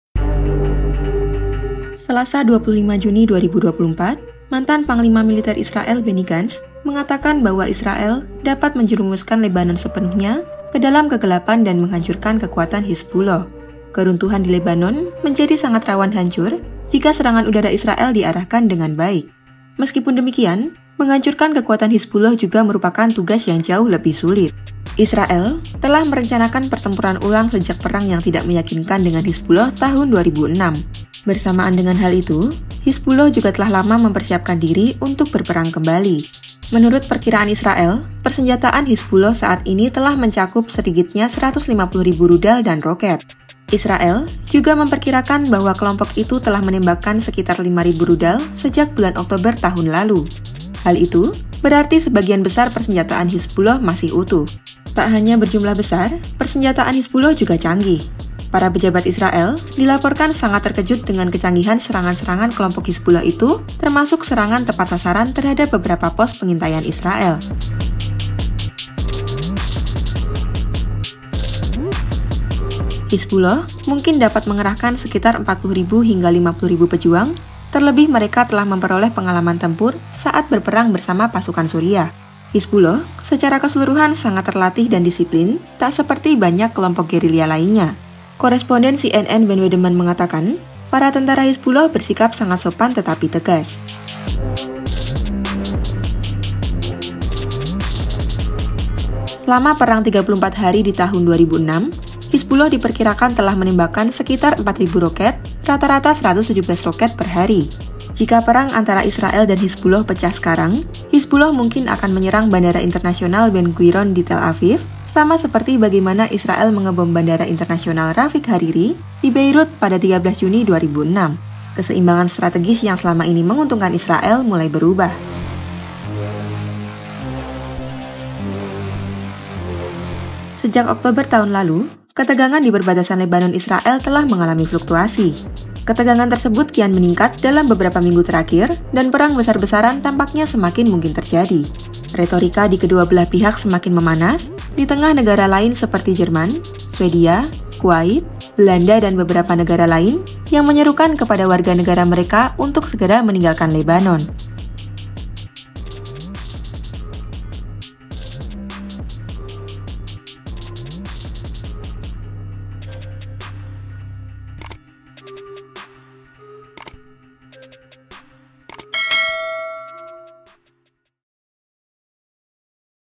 Perang Hizbullah-Israel Disebut Akan Jauh Lebih Berbahaya
Pada Selasa (25/6/2024), Benny Gantz mengatakan bahwa Israel dapat menjerumuskan Lebanon sepenuhnya ke dalam kegelapan dan menghancurkan kekuatan Hizbullah. Keruntuhan di Lebanon menjadi sangat rawan hancur, jika serangan udara Israel diarahkan dengan baik.

Meskipun demikian, menghancurkan kekuatan Hizbullah juga merupakan tugas yang jauh lebih sulit.

Penulis: Paramita Amaranggana
Penulis naskah: Katarina Astriyani Setyaningsih
Narator: Katarina Astriyani Setyaningsih
Video editor: Dimas Septian Adiyathama
Produser: Monica Arum